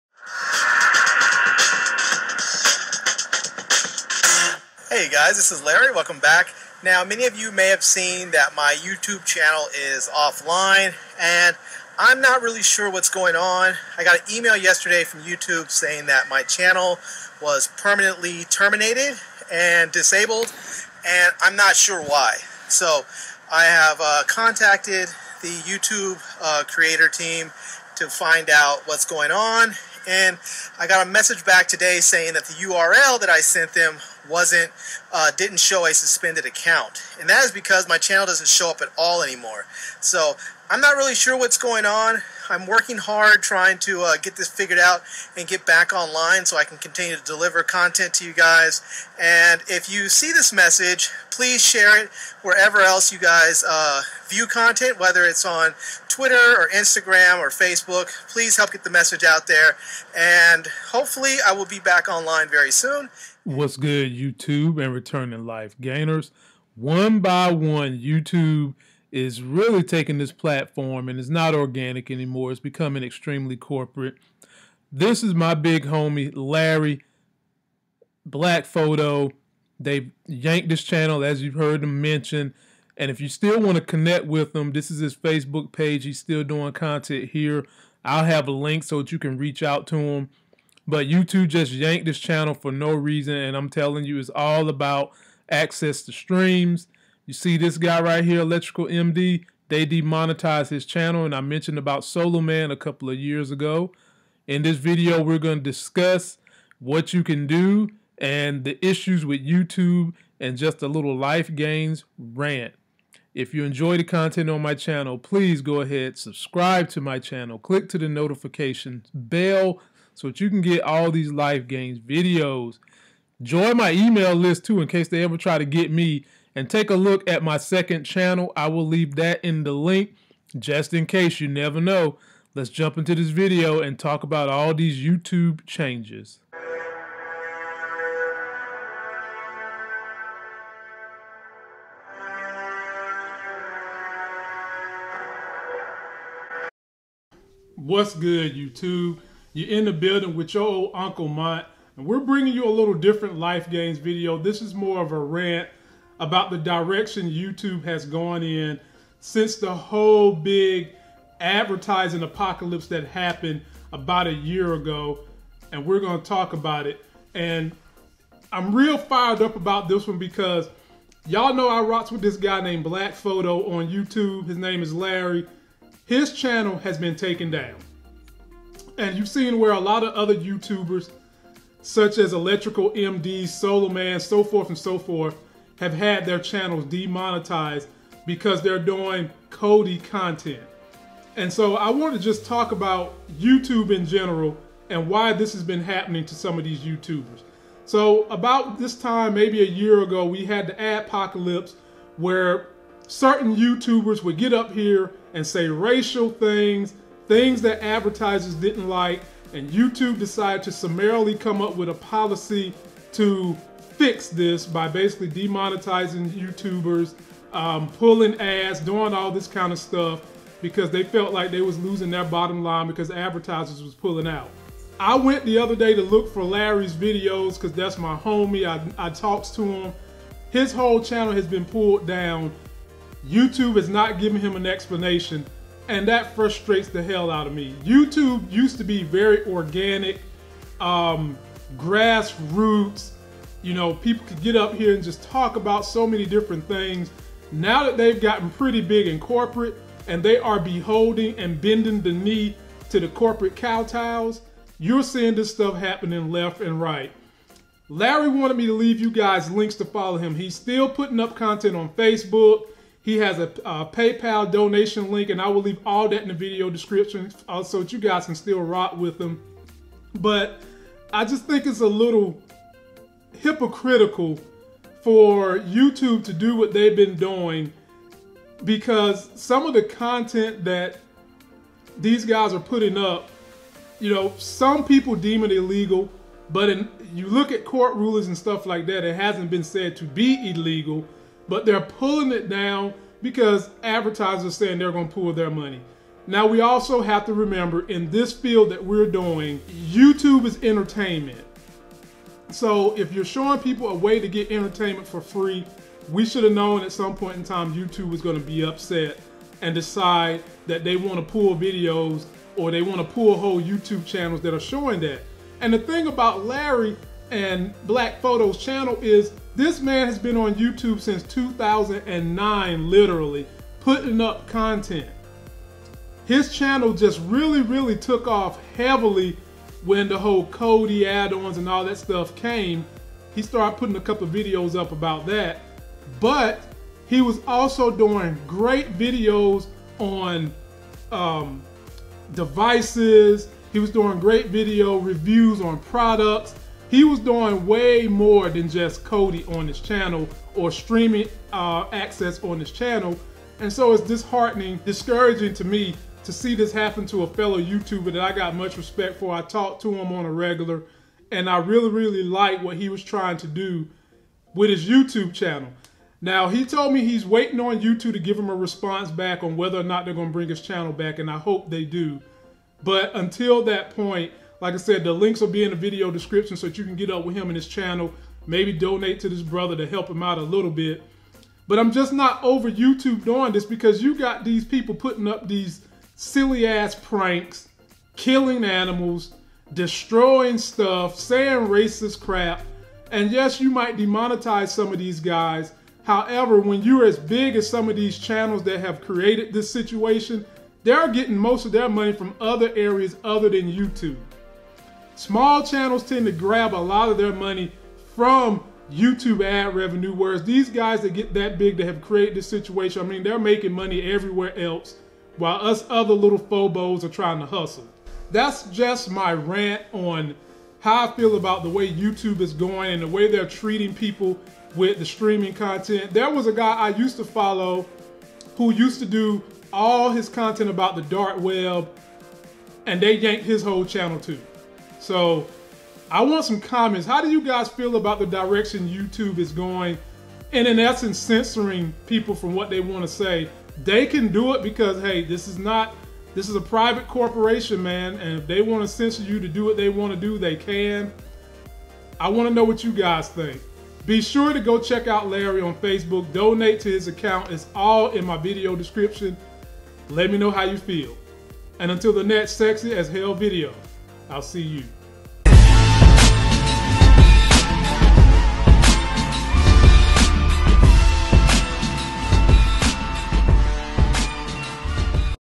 Kodi Youtube Channel Removed|No Warnings|Youtube is removing Channels/Demonetizing Them Over Streams
One of my favorite youtube channels was removed for Kodi content. We have also seen an increase in youtubers being demonetized for Kodi add-ons and streaming content.
Kodi youtube channel removed with no warnings and we discuss in this video.

To try the Best new IPTV service free for 24 hours